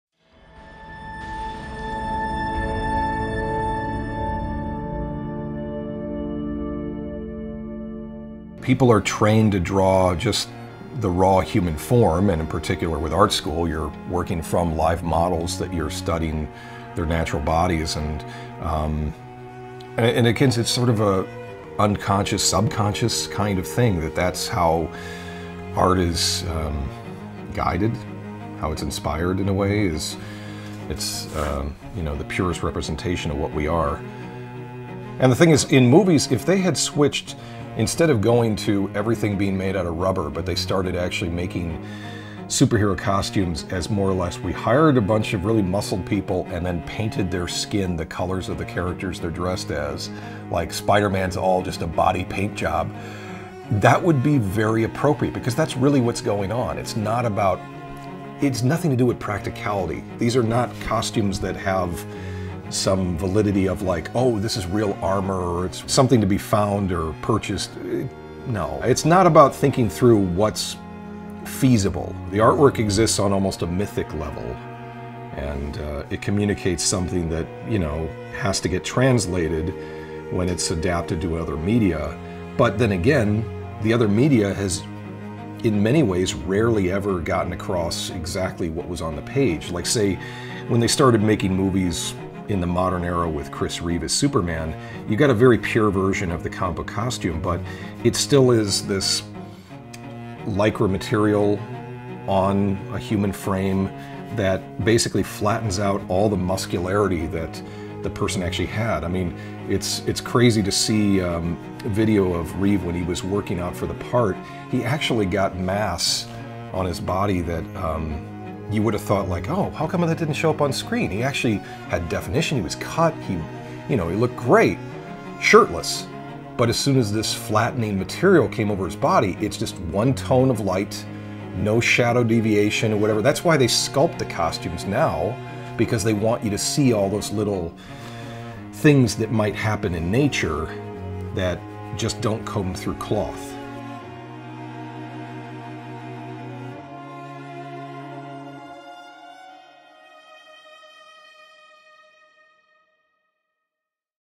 Comic Book Costumes on Film
Alex discusses comic book costumes on film